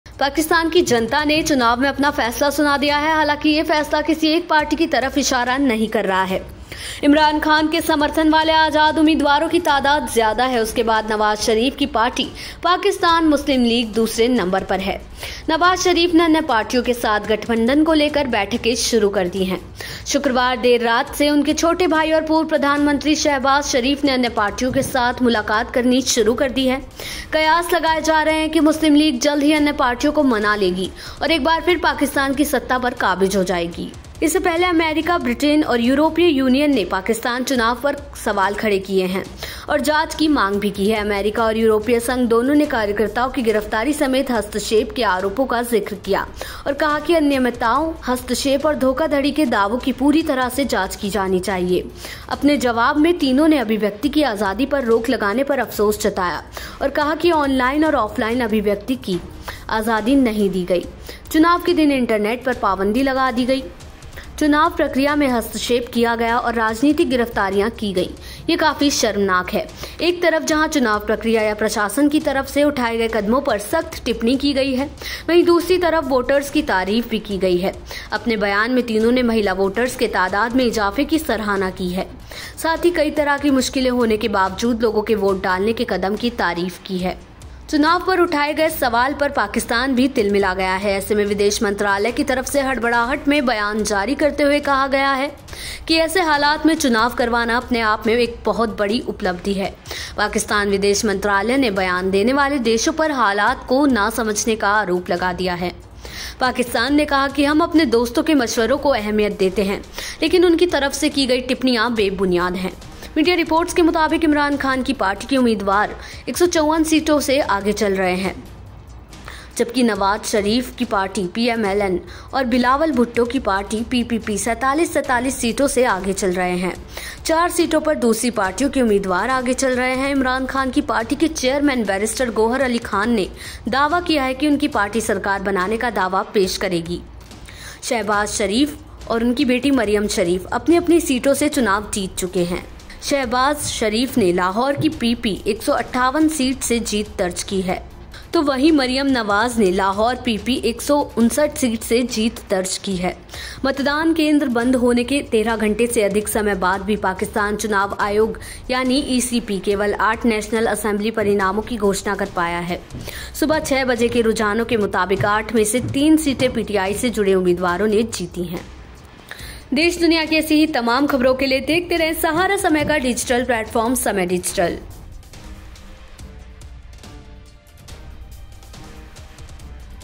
Pakistan Elections 2024: Pakistan में होने वाले चुनाव पर America मीडिया ने उठाए सवाल | World News
शुक्रवार देर रात से उनके छोटे भाई और पूर्व प्रधानमंत्री शाहबाज शरीफ ने अन्य पार्टियों के साथ मुलाकात करनी शुरू कर दी है. कयास लगाए जा रहे हैं कि मुस्लिम लीग जल्द ही अन्य पार्टियों को मना लेगी और एक बार फिर पाकिस्तान की सत्ता पर काबिज हो जाएगी।साथ ही कई तरह की मुश्किलें होने के बावजूद लोगों के वोट डालने के कदम की तारीफ़ की है।
Since late Friday night, his younger brother and former Prime Minister Shahbaz Sharif has started meeting with other parties. It is being speculated that the Muslim League will soon convince other parties and once again come to power in Pakistan. People have also been praised for their step of voting despite facing many difficulties.